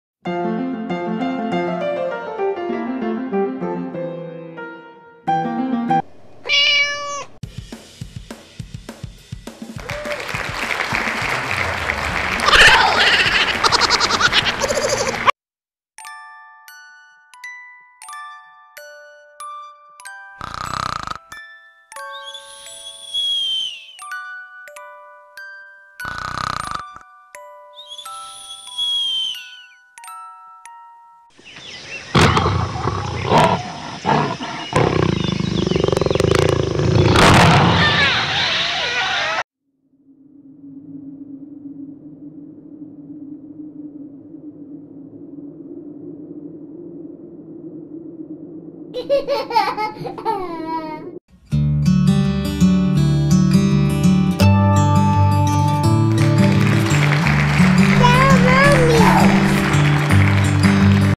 Can the cat play the piano?
I recommend you to watch the pianist cat lucia play the piano successfully.